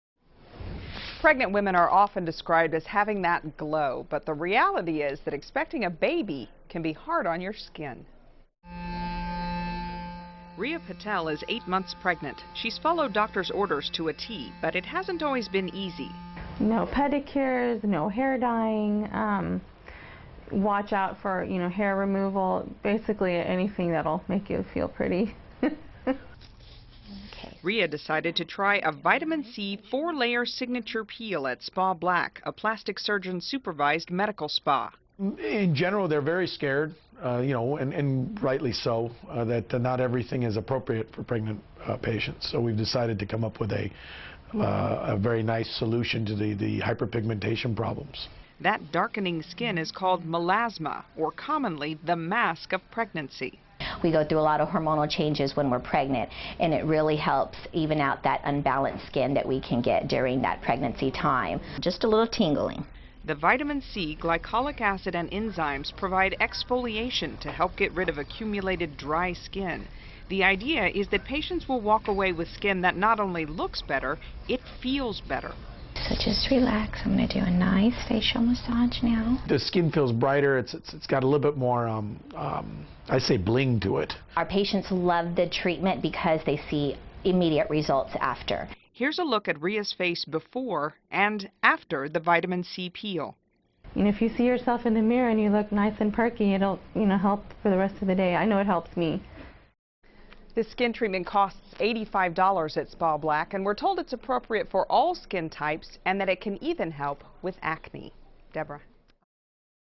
Dr Thomas Jeneby KENS 5 Spa Black Vitamin C Face Peel
Spa Black offers women a four layer vitamin c face peel. Patients will see immediate results. Dr. Jeneby introduced this as a safe way for pregnant women to look and feel better. This peel also helps women with acne and is safe for all skin types